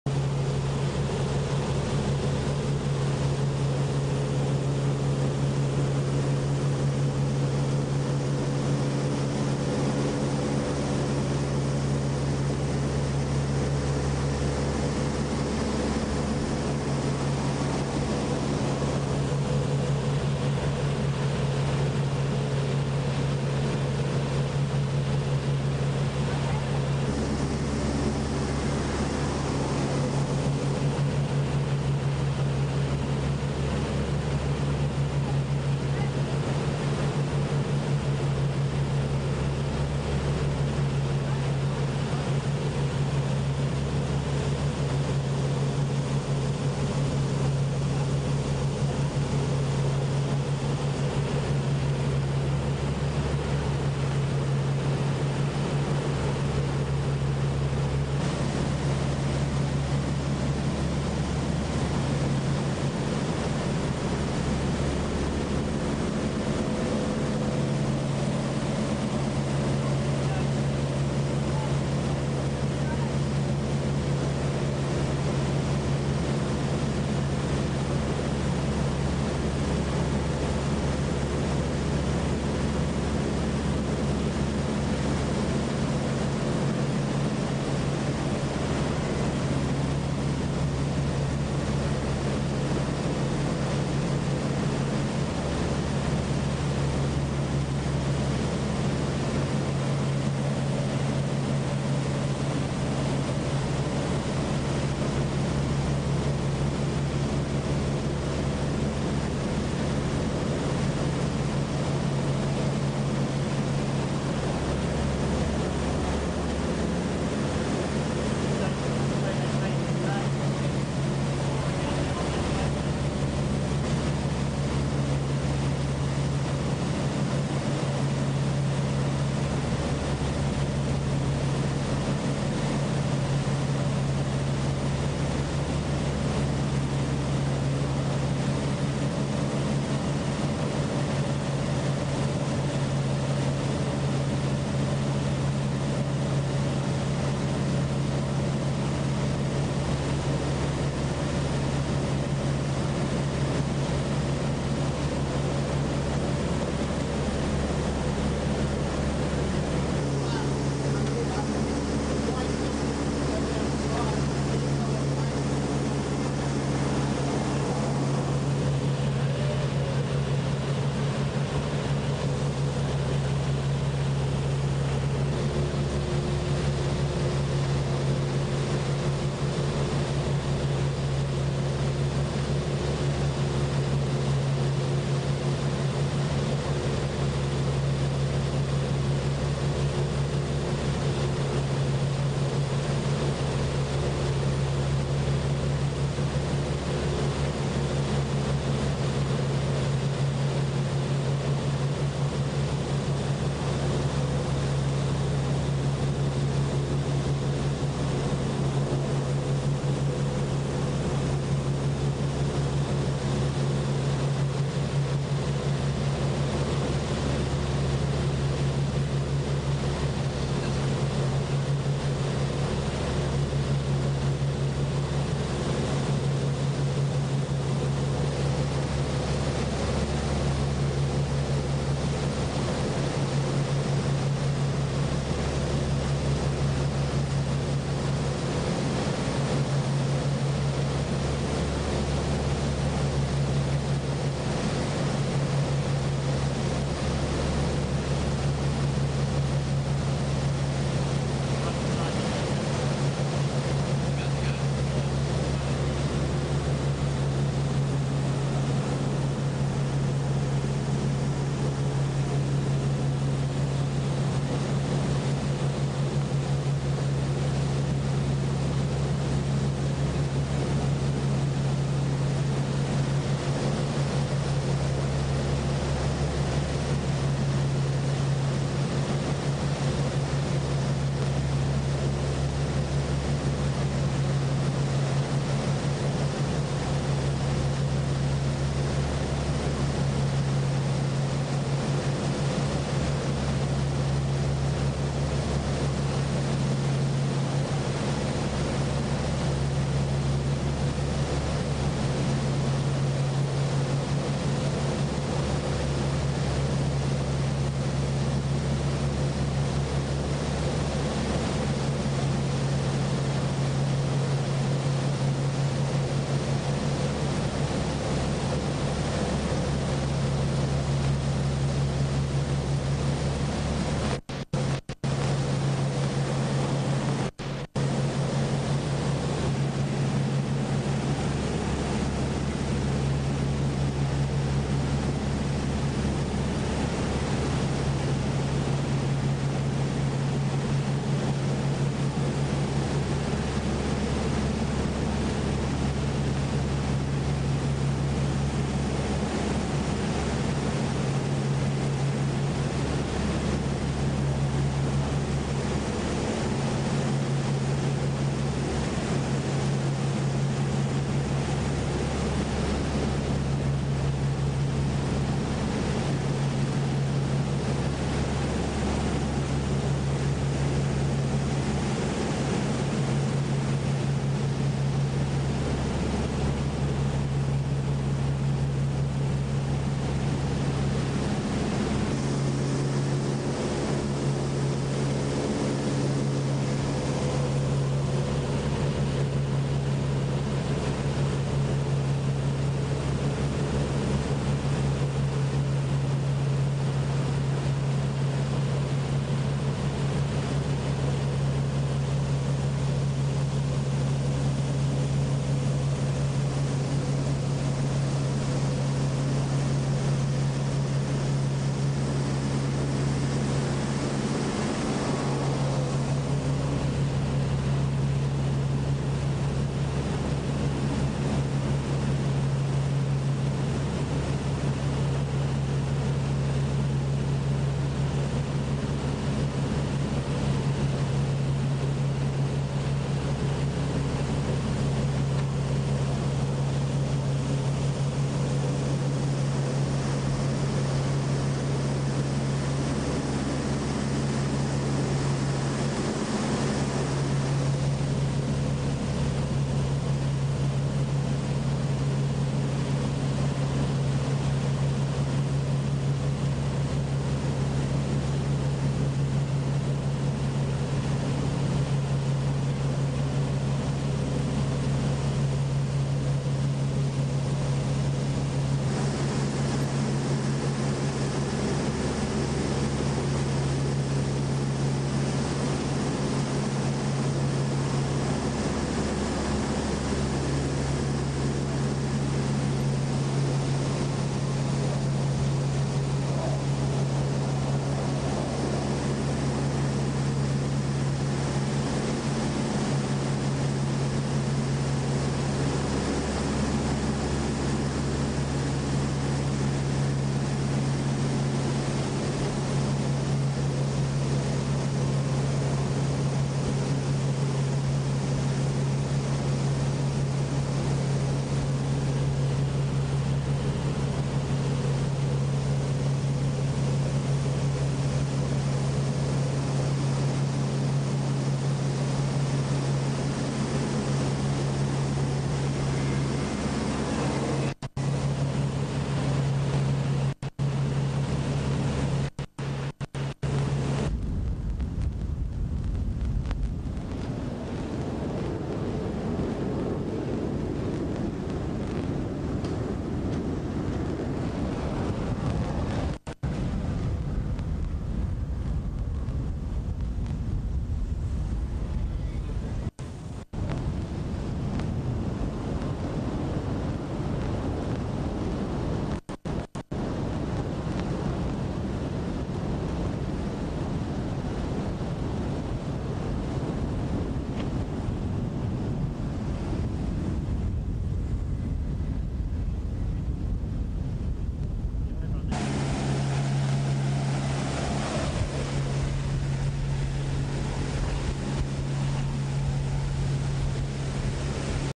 Sean and Andy Jet Ski White Mischief
Sean and Andy Jet skiing and wake jumping White Mischief in Auckland harbour beautiful perfect calm summer day. Shirley Waru with us.  Thanks to John Wrightson for the boat and the film crew